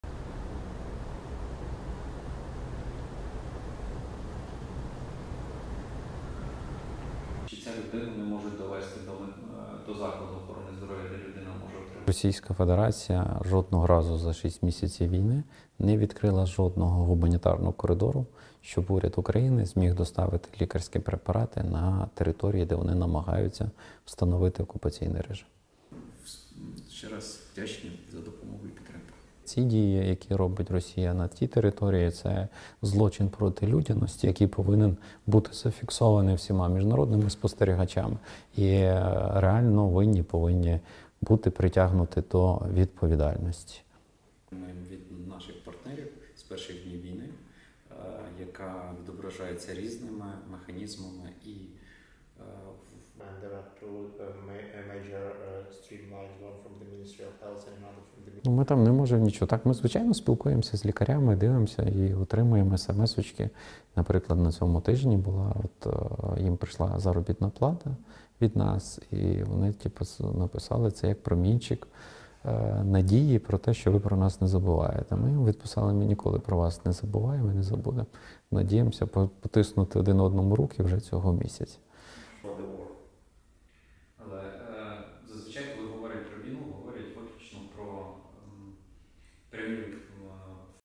Ukrainian minister: Russia blocking access to medicines
RESTRICTION SUMMARY:
ASSOCIATED PRESS
Kyiv - 12 August 2022
1. Exterior of Ministry of Health of Ukraine building
2. Sign on wall reads, (Ukrainian): "Ministry of Health of Ukraine"
3. Viktor Liashko, Ukraine's Minister of Health, speaking with AP journalists
4. SOUNDBITE: (Ukrainian) Viktor Liashko, Minister of Health of Ukraine:
"Throughout the entire six months of war, Russia has not (allowed) proper humanitarian corridors so we could provide our own medicines to the patients that need them."
5. Liashko's hands
6. SOUNDBITE: (Ukrainian) Viktor Liashko,  Minister of Health of Ukraine:
"We believe that these actions are being taken with intent by Russia and we consider them to be crimes against humanity and war crimes that will be documented and will be recognized."
7. Wide of Liashko speaking
8. Camera filming interview
9. SOUNDBITE: (Ukrainian), Viktor Liashko,  Minister of Health of Ukraine:
"We can't do anything there (in the occupied territories). This is how we usually communicate with doctors, look and receive SMS. For example, this week they received their salary from us. And they wrote to us that it's like a ray of hope that you don't forget about us. We wrote that we have never forgotten you and will not forget you and hope to shake hands this month."
10. Liashko speaking with AP journalists
STORYLINE
Ukraine's health minister has accused Russian authorities of blocking access to affordable medicines in areas in if the country it has occupied since the invasion launched on Febuary 24 ‒ describing the action as a "crime against humanity".
In an interview with the Associated Press, Viktor Liashko said efforts to provide state subsidized drugs to people in the Russian-held territories had been repeatedly rejected.
The program provides patients with cancer and chronic diseases medicines that are fully reimbursed by the Ukrainian government.
The war in Ukraine has caused severe disruptions to the country's state-run health service which had been undergoing major reforms largely in response to the pandemic.
The destruction of hospitals and infrastructure along with the displacement of several million people inside the country have caused major disruptions to treatment plans.
The World Health Organization says it has recorded 445 attacks on hospitals and other health care facilities, between Feb. 24 and Aug. 11, with the direct result of 86 deaths and 105 injuries.
But Liashko said the secondary effects are far more severe.